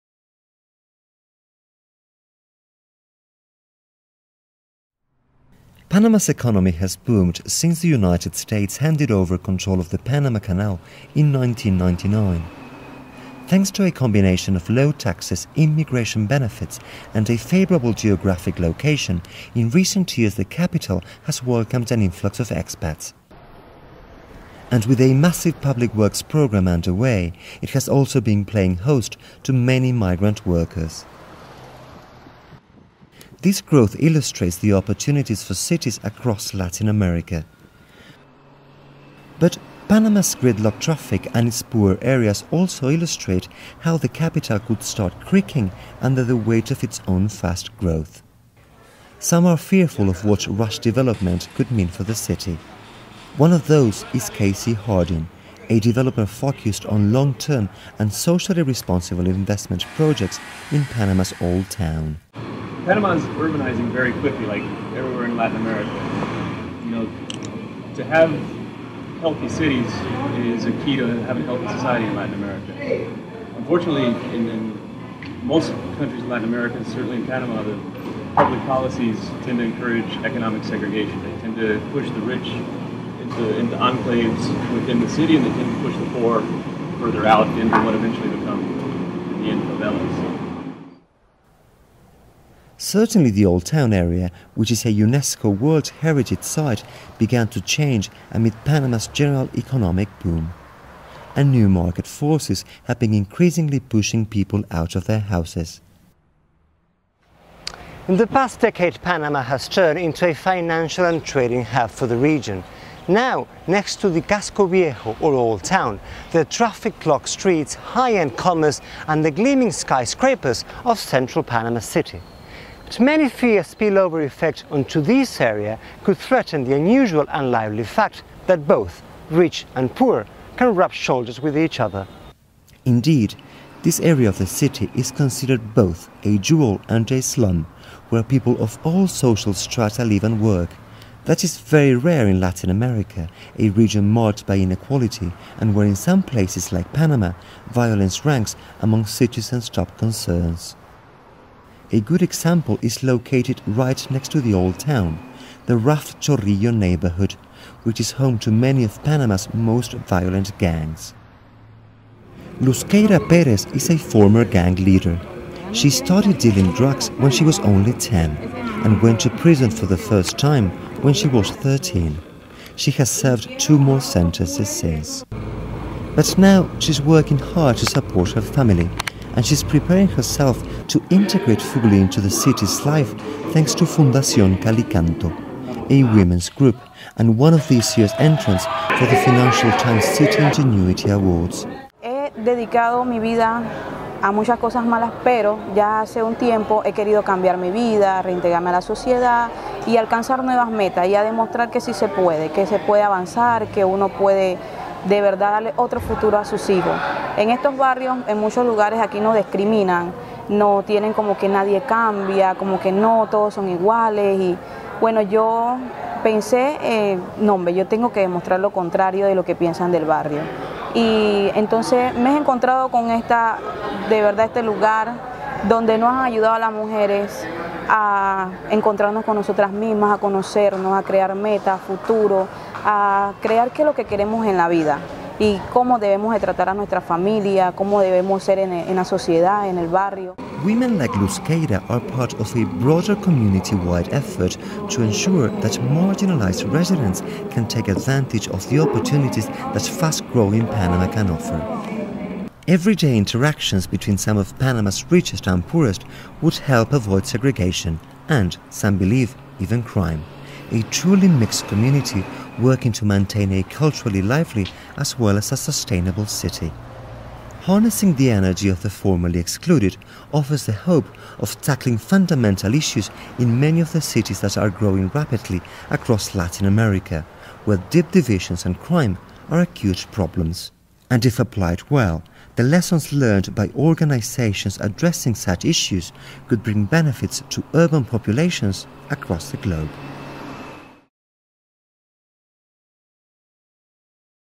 Latin America's urban challenge
Panama City offers the hope of resisting Latin America's trend towards urban polarisation, reports Andres Schipani